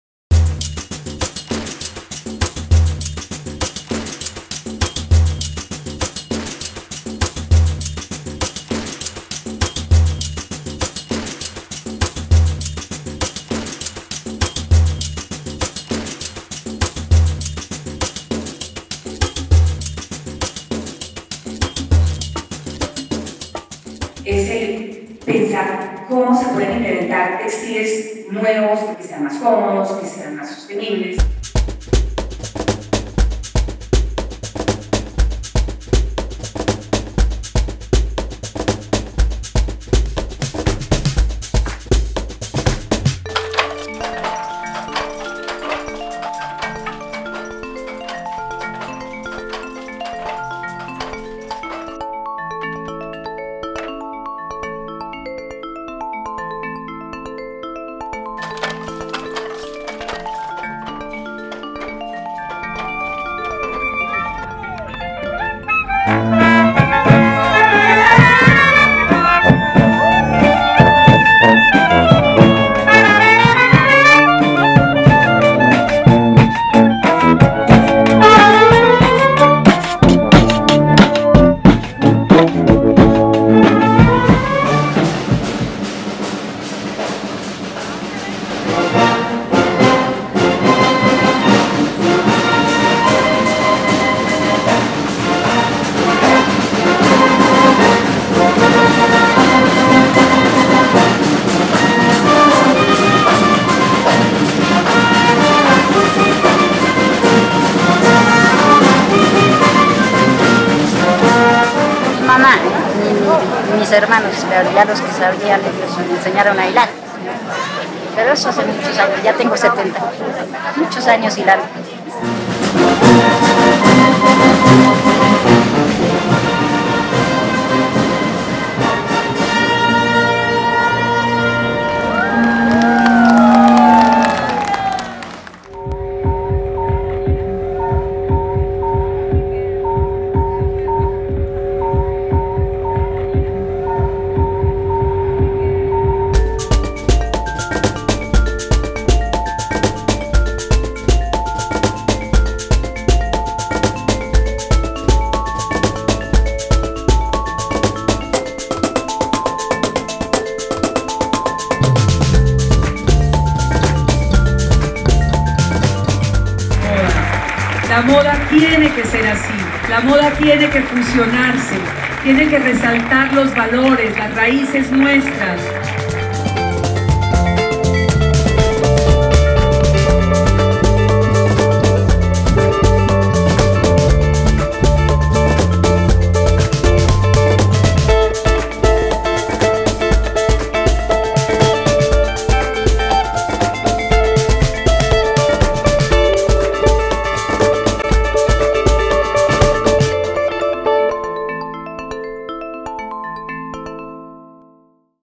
FESTILANA 2014
Este año el festival de la lana que se realiza en Cucunubá contó con más de 1000 asistentes, fue la primera vez que se realizó dos días y la pasarela de moda sorprendió a más de uno con las colecciones de cinco diseñadores colombianos y de la invitada internacional María Clara Restrepo y su marca ROHKA. Además el festival recibió el aval de la Campaña de la lana por parte de su Alteza real el Príncipe de Gales durante su visita a Bogotá.